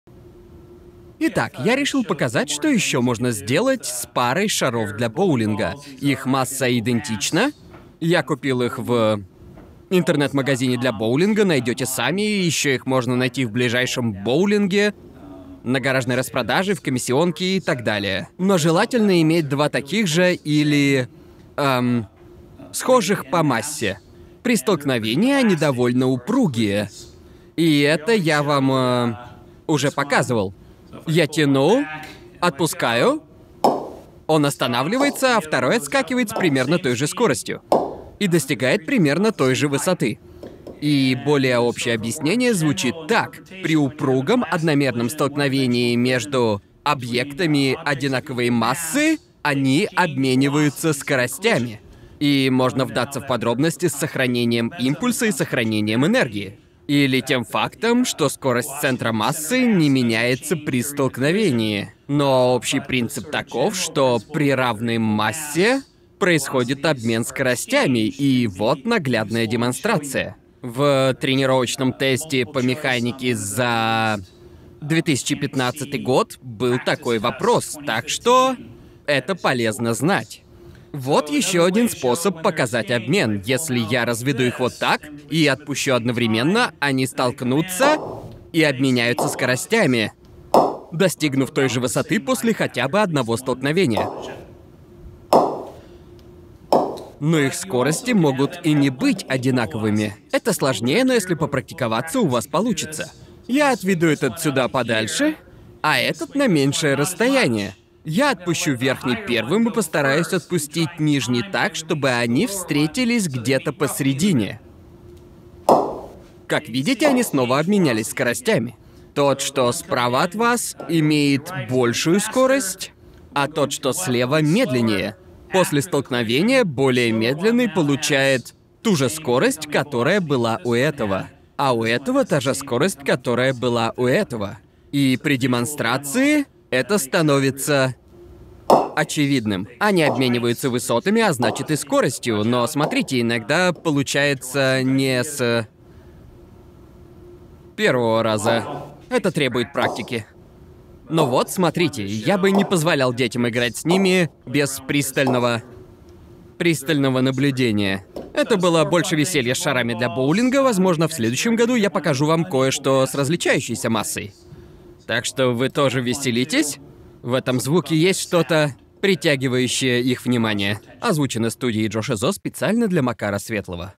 Упругие столкновения шаров для боулинга
Демонстрации упругих столкновений равной массы с использованием шаров для боулинга. Можно заметить, что при одномерных упругих столкновениях объекты равной массы обмениваются скоростями.